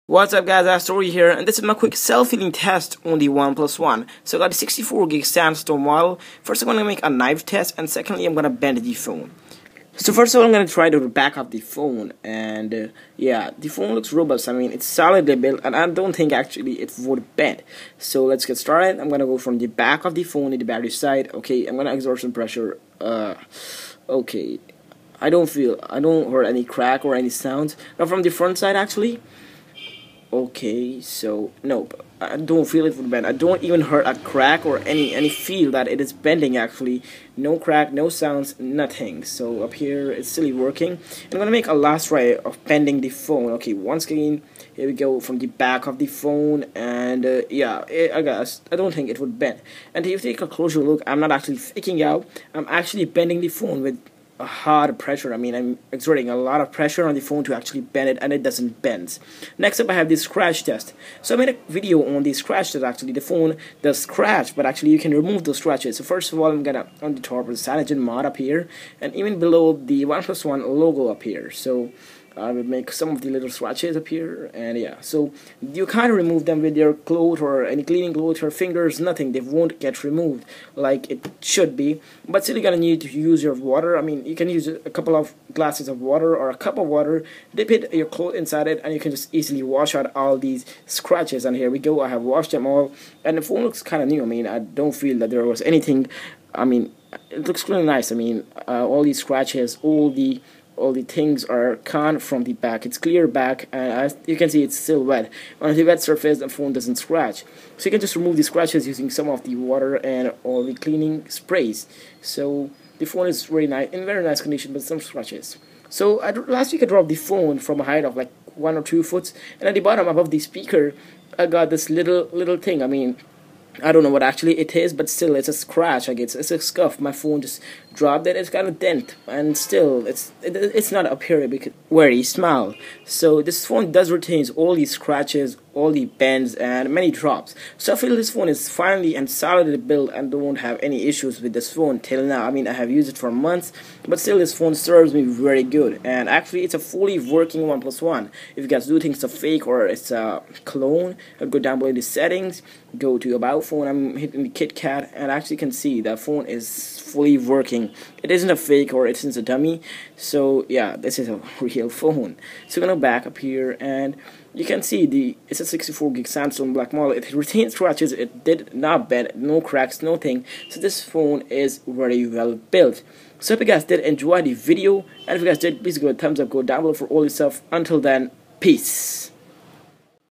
OnePlus One Bend Test!
Here is my Quick Self Healing & Bend Test of OnePlus One!!

Get The Wallpaper From Here - justinmaller.com or facets.la